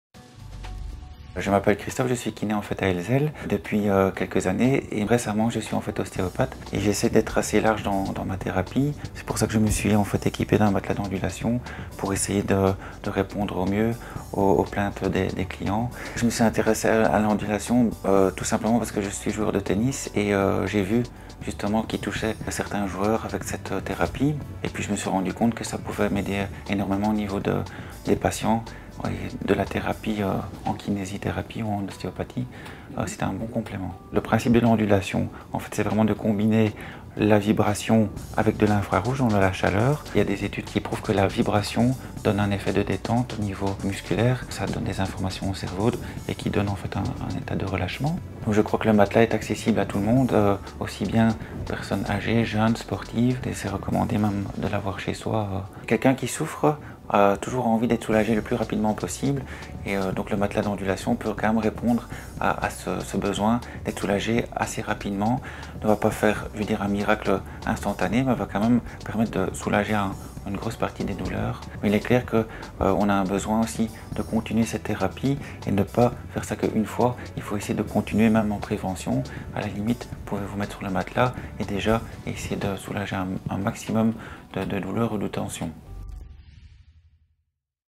Rugpijn Andullatie HHP
Christophe Mahieu is kinesist en osteopaat in Ellezelles. Hij vat zijn 'therapie' op in de brede zin van het woord. Daarom heeft hij ook een andullatiematras aangeschaft. De bedoeling is zo goed mogelijk tegemoet te komen aan de klachten van zijn klanten. Zijn interesse voor andullatie werd gewekt vanuit het tennismilieu. Maar hij begreep al snel dat de matras hem ook grote diensten kon bewijzen in zijn praktijk. Andullatie is een goede aanvulling bij kinesitherapie en osteopathie. Het is gebaseerd op een combinatie van vibraties en infrarood. Studies toonden aan dat vibraties een relaxerend effect hebben op de spieren. In feite wordt er informatie doorgegeven naar de hersenen, die dan een toestand van relaxatie oproepen. Volgens Christophe is iedereen gebaat met andullatie, zowel ouderen, jongeren als sportlui. Het is ook handig een matras in huis te hebben. Al wie pijn heeft wil hier zo snel mogelijk van verlost zijn. De andullatiematras kan hier goede diensten leveren. Het is geen mirakeltherapie, maar iedereen slaagt er toch in zijn pijn grotendeels weg te werken. Let wel: het is een therapie die blijvend toegepast moet worden. Eén keer volstaat dus niet.